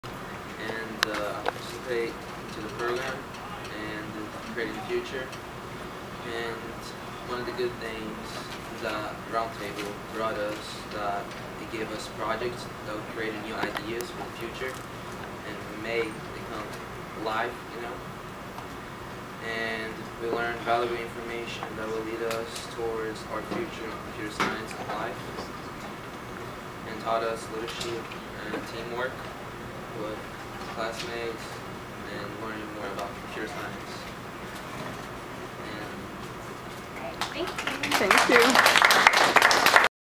Luan Agostinho.avi
Luan Agostinho, rising 12th grader, giving his reflections on the program and computing.